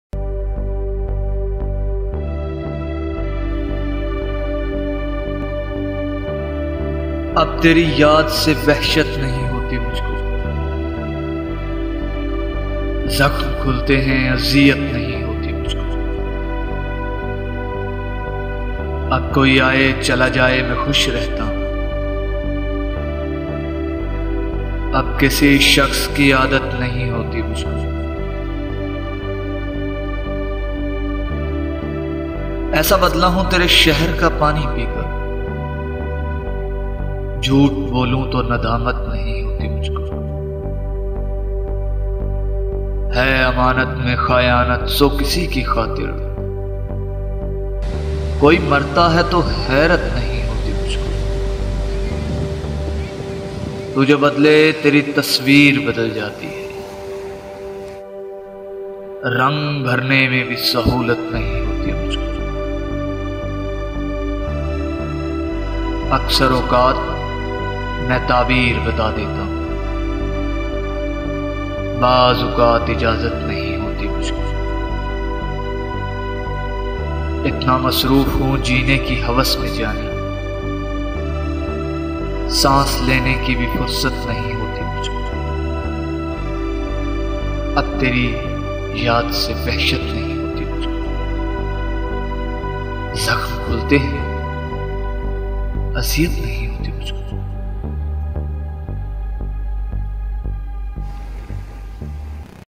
Sad poetry in urdu new 2020 - Ab Teri Yaad Se Wahshat Nahi Hoti Mujhko -Best Urdu Ghazal
This is a really heart touching urdu poetry this poetry  is a heart breaking i love this urdu shayari. This urdu poetry voice by zubair jani and edit by  zs poetry.new shayari 2020 is so sad and so emotional.i love this urdu poetry with music and very bieautiful video.Sad poetry in urdu new 2020 - Ab Teri Yaad Se Wahshat Nahi Hoti Mujhko -Best Urdu Ghazal
Voice By...Zubair jani
Editting By...Zubair jani
From Okara Pakistan